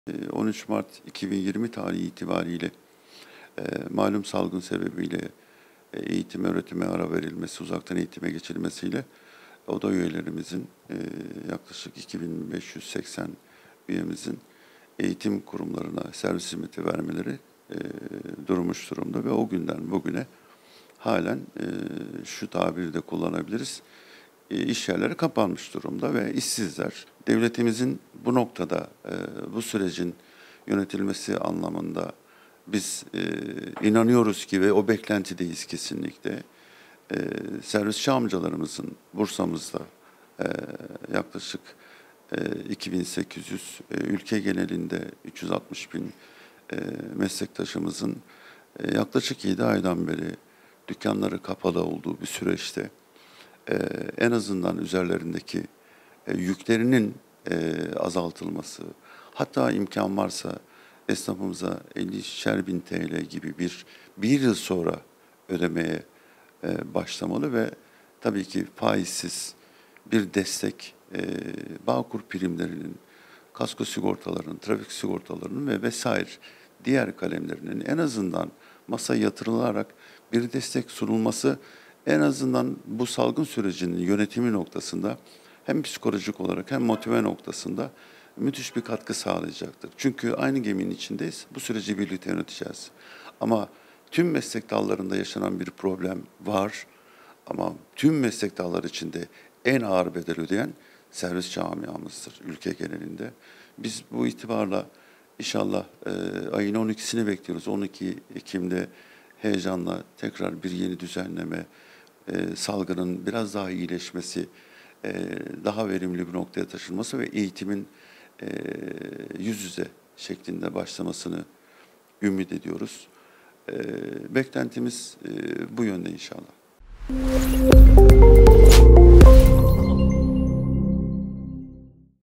Bursa'da okul servisçileri acil destek bekliyor!
Korona virüs salgınının tüm hızıyla devam etmesi nedeniyle tam anlamıyla başlanılamayan yüz yüze eğitim, bir çok sektörü de zorda bıraktı. Kantinciler ve kırtasiyeciler kadar okul servisleri de olumsuz etkilendi.